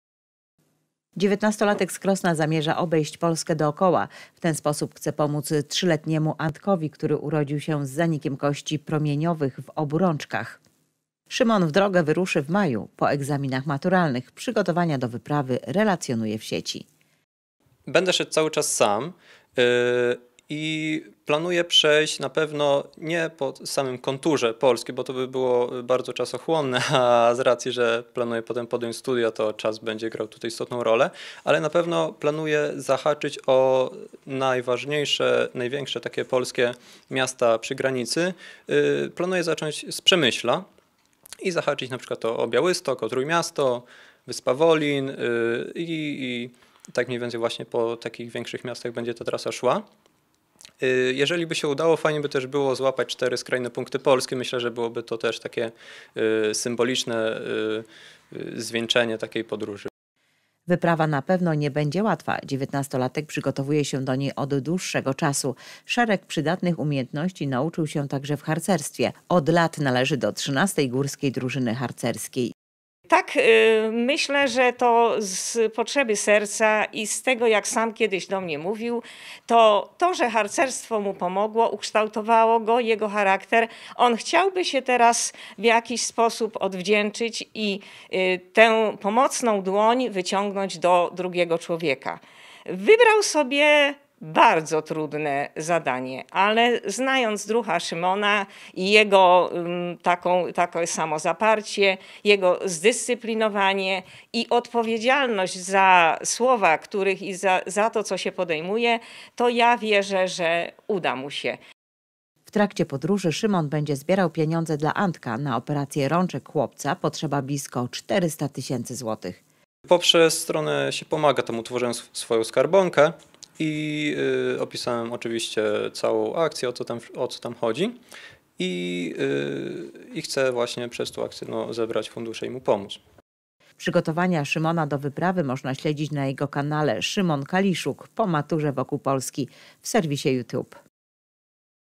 Po Maturze Wokół Polski
19-latek z Krosna zamierza obejść Polskę dookoła. W ten sposób chce pomóc 3-letniemu Antkowi, który urodził się z zanikiem kości promieniowych w obu rączkach. Szymon w drogę wyruszy w maju, po egzaminach maturalnych. Przygotowania do wyprawy relacjonuje w Sieci.